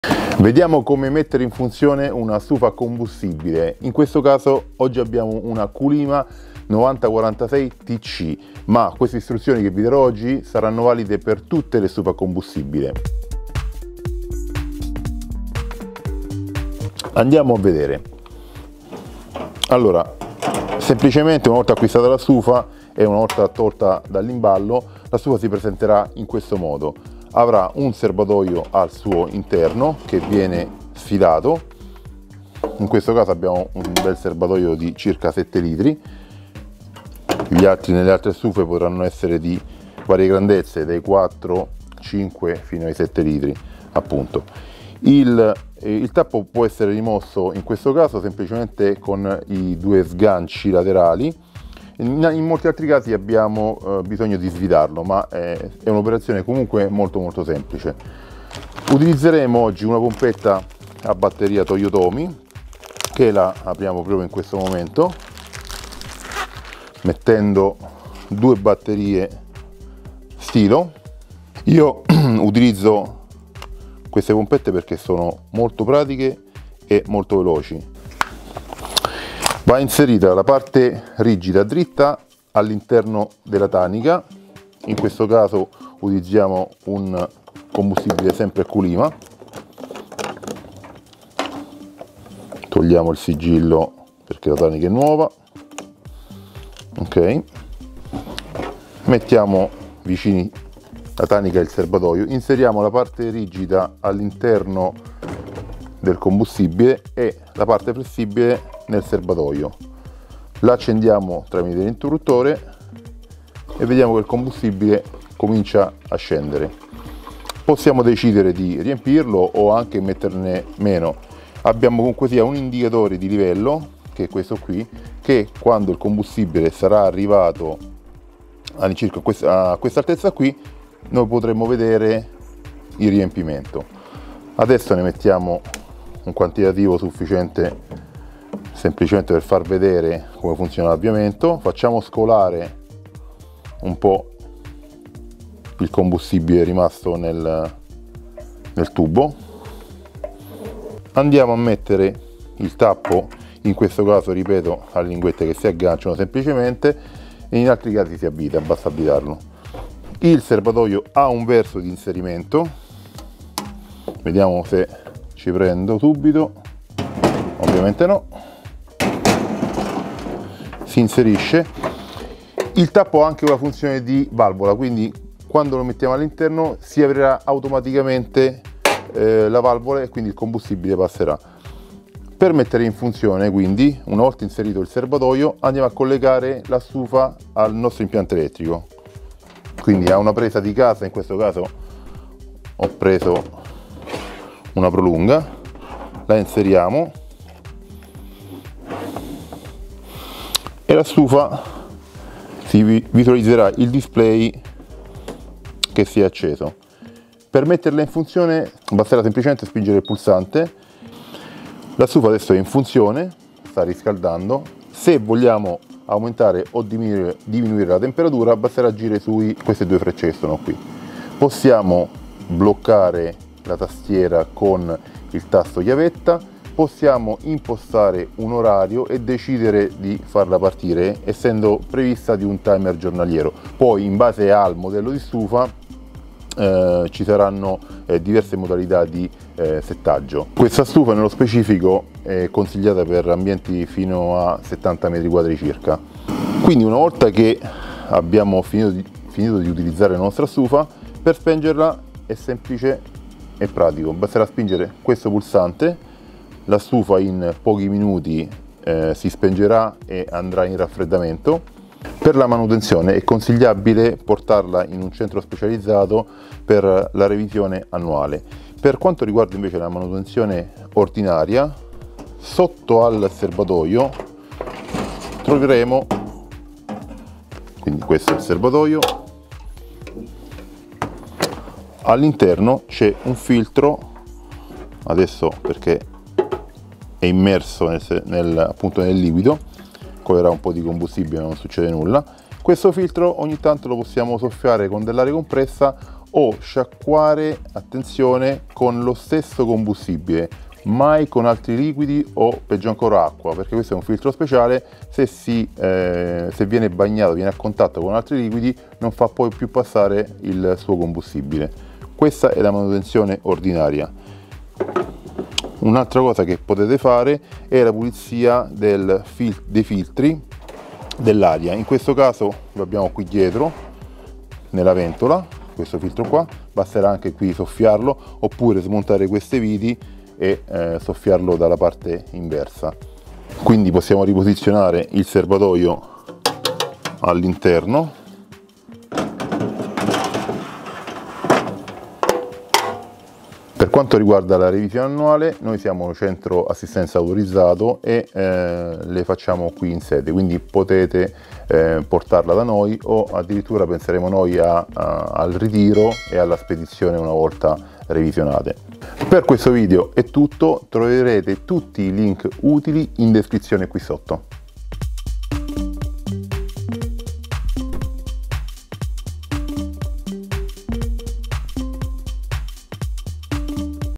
Prima accensione stufa a combustibile + Manutenzione ordinaria
In questo video tutorial, andremo a vedere la messa in funzione  della stufa a combustibile!

👇Indice👇
00:00-00:19 introduzione
00:19-00:59 Estrazione serbatoio
00:59-03:02 Inserimento combustibile
03:00-04:06 Spegnimento
04:06-04:22 Manutenzione ordinaria (filtro e ventola)
04:00-05:44 Revisione

Le stufe sono una valida alternativa al gas per scaldarsi, andando a risparmiare sul gas e sulla corrente.

📌Via Lago di Como 2, Fonte Nuova (RM)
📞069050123